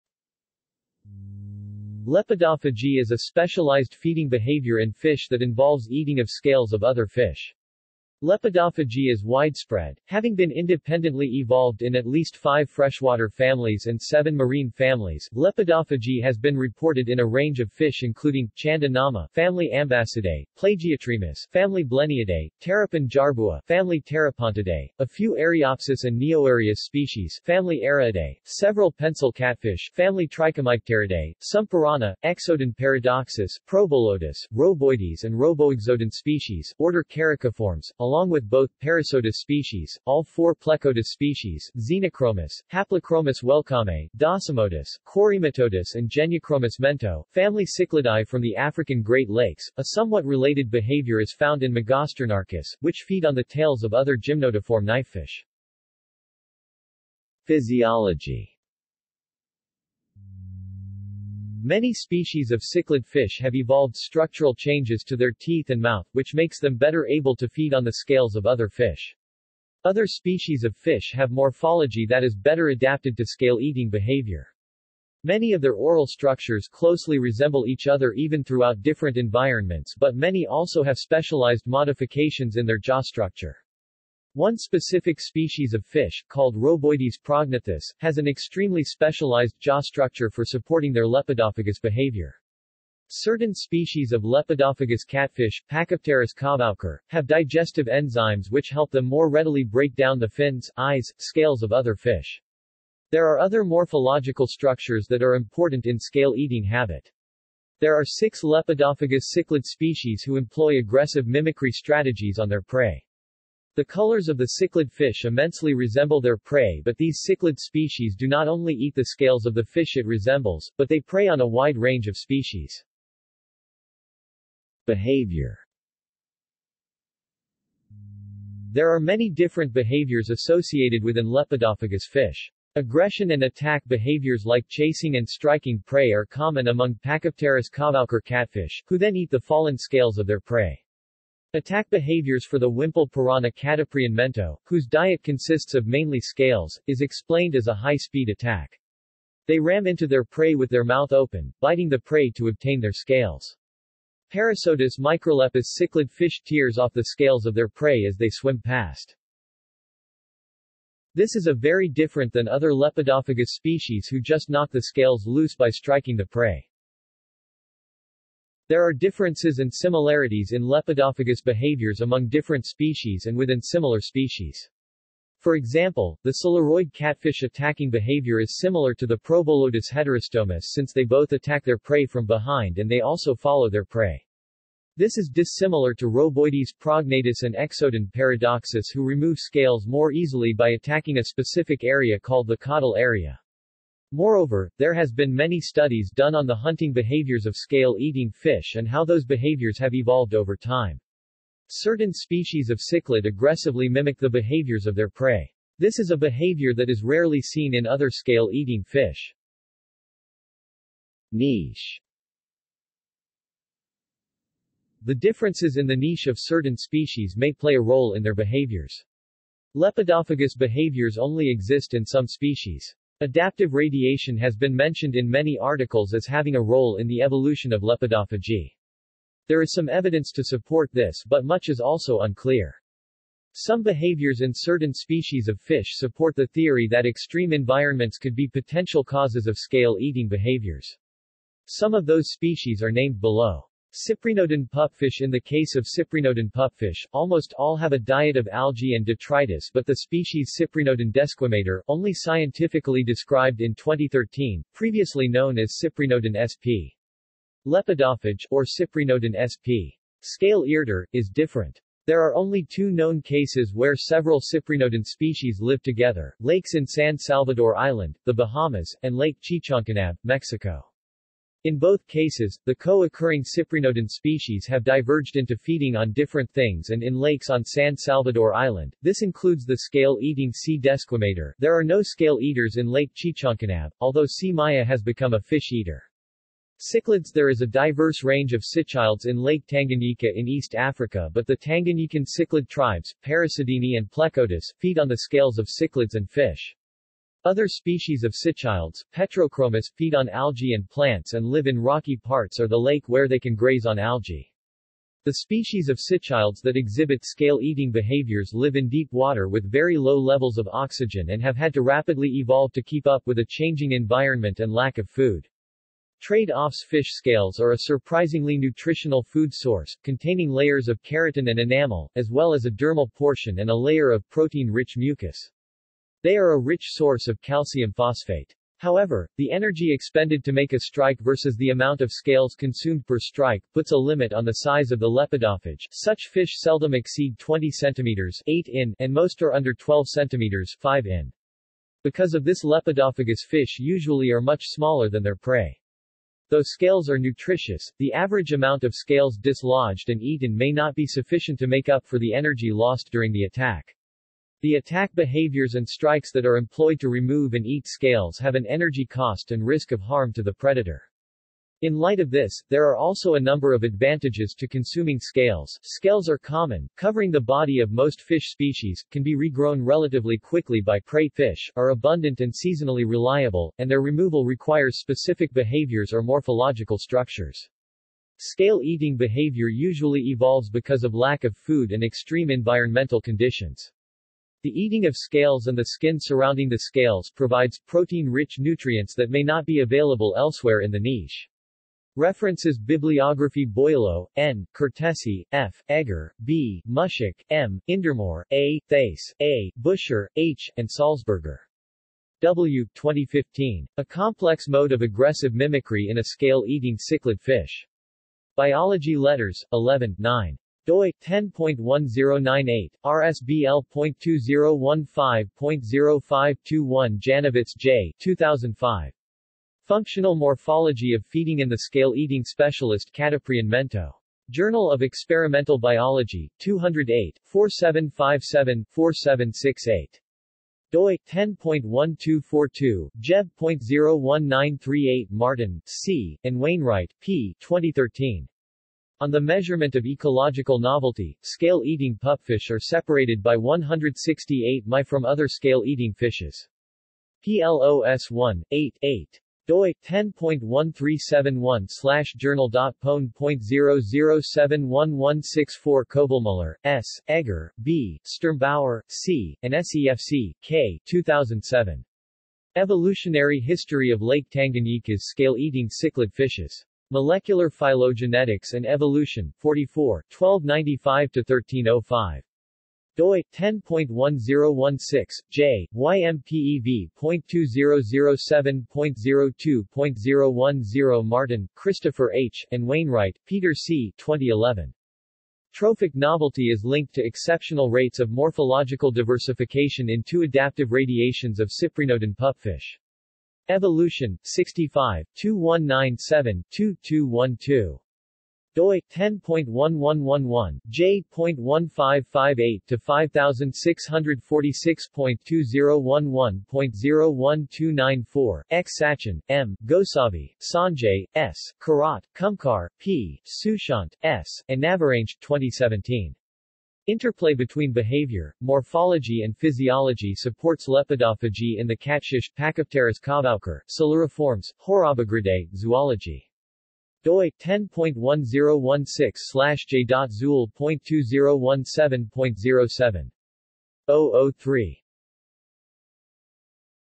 Lepidophagy | Wikipedia audio article
This is an audio version of the Wikipedia Article:
Lepidophagy

SUMMARY
Lepidophagy is a specialised feeding behaviour in fish that involves eating of scales of other fish. Lepidophagy is widespread, having been independently evolved in at least five freshwater families and seven marine families.Lepidophagy has been reported in a range of fish including: Chanda nama (family Ambassidae), Plagiotremus (family Blenniidae), Terapon jarbua (family Terapontidae), a few Ariopsis and Neoarius species (family Ariidae), several pencil catfish (family Trichomycteridae), some piranha, Exodon paradoxus, Probolodus, Roeboides and Roeboexodon species (order Characiformes), along with both Perissodus species, all four Plecodus species, Xenochromis, Haplochromis welcommei, Docimodus, Corematodus and Genyochromis mento (family Cichlidae from the African Great Lakes).A somewhat related behavior is found in Magosternarchus, which feed on the tails of other gymnotiform knifefish.